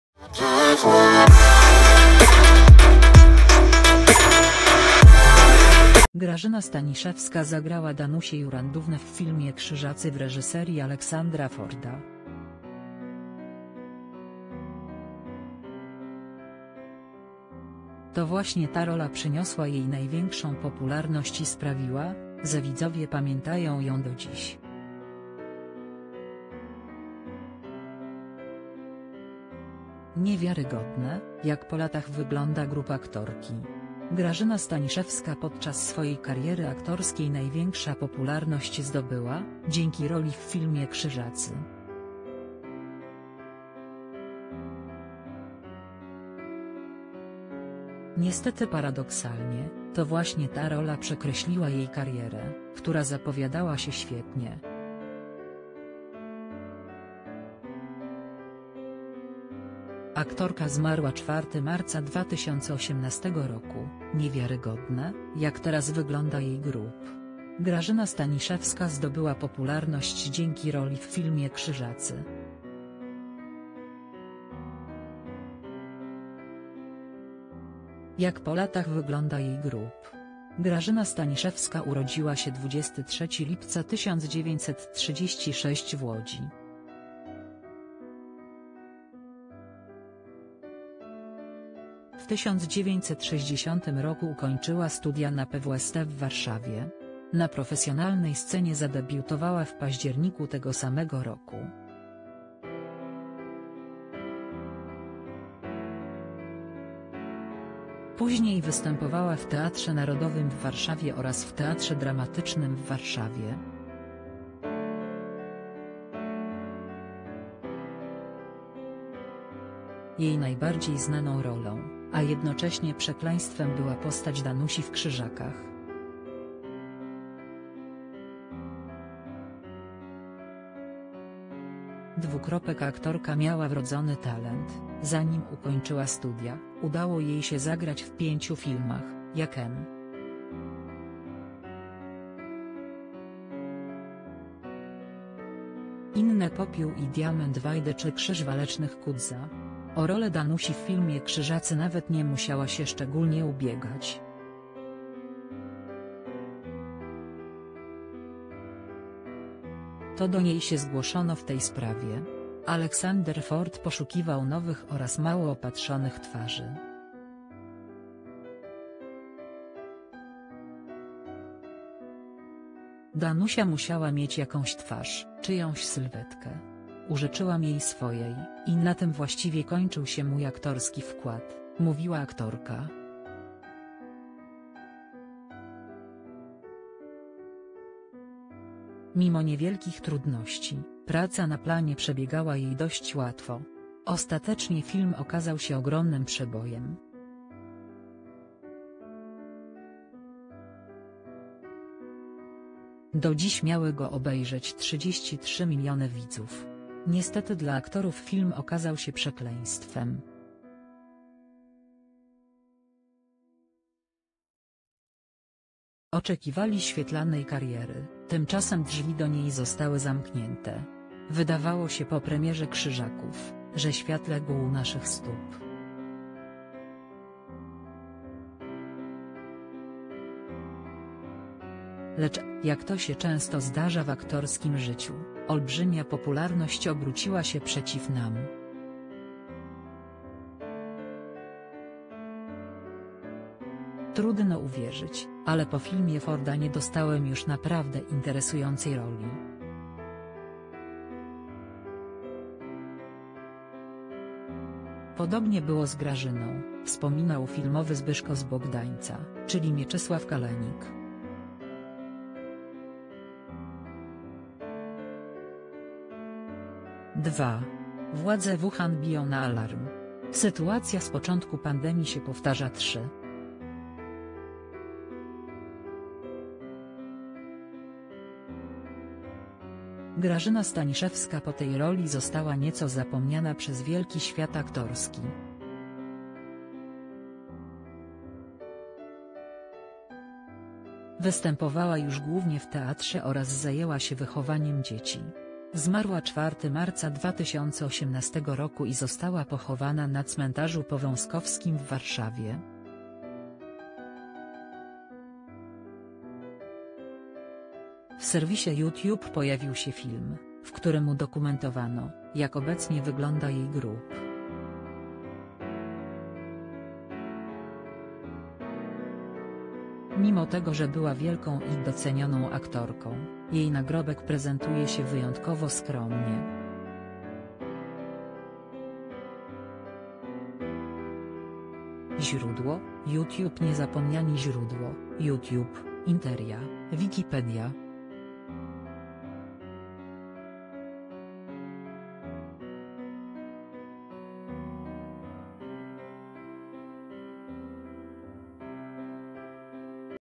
W "Krzyżakach" zagrała Danusię. Niewiarygodne, jak dzisiaj wygląda grób Grażyny Staniszewskiej
Grażyna Staniszewska zagrała Danusię Jurandównę w filmie "Krzyżacy" w reżyserii Aleksandra Forda. To właśnie ta rola przyniosła jej największą popularność i sprawiła, ze widzowie pamiętają ją do dziś. Niewiarygodne, jak po latach wygląda grób aktorki.   Grażyna Staniszewska podczas swojej kariery aktorskiej największa popularność zdobyła, dzięki roli w filmie "Krzyżacy". Niestety paradoksalnie, to właśnie ta rola przekreśliła jej karierę, która zapowiadała się świetnie. Aktorka zmarła 4 marca 2018 roku, niewiarygodne, jak teraz wygląda jej grób.      Grażyna Staniszewska zdobyła popularność dzięki roli w filmie "Krzyżacy". Jak po latach wygląda jej grób?  Grażyna Staniszewska urodziła się 23 lipca 1936 w Łodzi. W 1960 roku ukończyła studia na PWST w Warszawie. Na profesjonalnej scenie zadebiutowała w październiku tego samego roku. Później występowała w Teatrze Narodowym w Warszawie oraz w Teatrze Dramatycznym w Warszawie. Jej najbardziej znaną rolą, a jednocześnie przekleństwem była postać Danusi w "Krzyżakach".   :      Aktorka miała wrodzony talent, zanim ukończyła studia, udało jej się zagrać w pięciu filmach, jak m.in. "Popiół i diament" Wajdy czy "Krzyż walecznych" Kutza. O rolę Danusi w filmie "Krzyżacy" nawet nie musiała się szczególnie ubiegać. To do niej się zgłoszono w tej sprawie. Aleksander Ford poszukiwał nowych oraz mało opatrzonych twarzy.   - Danusia musiała mieć jakąś twarz, czyjąś sylwetkę. Użyczyłam jej swojej, i na tym właściwie kończył się mój aktorski wkład - mówiła aktorka. Mimo niewielkich trudności, praca na planie przebiegała jej dość łatwo. Ostatecznie film okazał się ogromnym przebojem. Do dziś miały go obejrzeć 33 miliony widzów. Niestety dla aktorów film okazał się przekleństwem. Oczekiwali świetlanej kariery, tymczasem drzwi do niej zostały zamknięte.   - Wydawało się po premierze "Krzyżaków", że świat legł u naszych stóp. Lecz, jak to się często zdarza w aktorskim życiu, olbrzymia popularność obróciła się przeciw nam. Trudno uwierzyć, ale po filmie Forda nie dostałem już naprawdę interesującej roli. Podobnie było z Grażyną - wspominał filmowy Zbyszko z Bogdańca, czyli Mieczysław Kalenik.      2. Władze Wuhan biją na alarm. Sytuacja z początku pandemii się powtarza  3.   Grażyna Staniszewska po tej roli została nieco zapomniana przez wielki świat aktorski. Występowała już głównie w teatrze oraz zajęła się wychowaniem dzieci. Zmarła 4 marca 2018 roku i została pochowana na Cmentarzu Powązkowskim w Warszawie. W serwisie YouTube pojawił się film, w którym udokumentowano, jak obecnie wygląda jej grób. Mimo tego że była wielką i docenioną aktorką, jej nagrobek prezentuje się wyjątkowo skromnie.       Źródło: YouTube Niezapomniani      Źródło: YouTube, Interia, Wikipedia